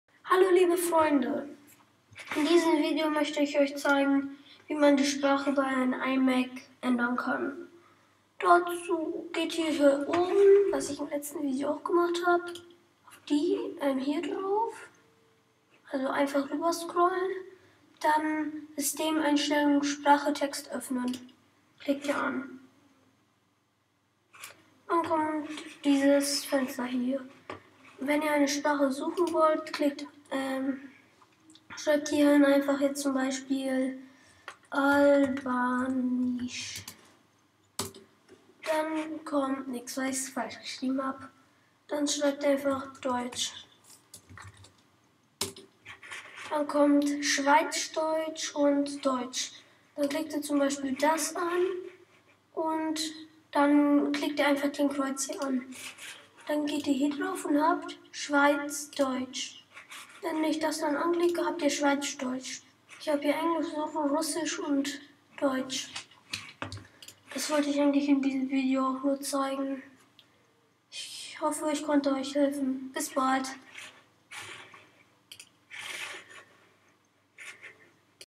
Sprache ändern (Mac)
In diesen Video zeige ich euch wie ihr bei einem Mac (iMac),
die Sprache ändern könnt !      viel erfolg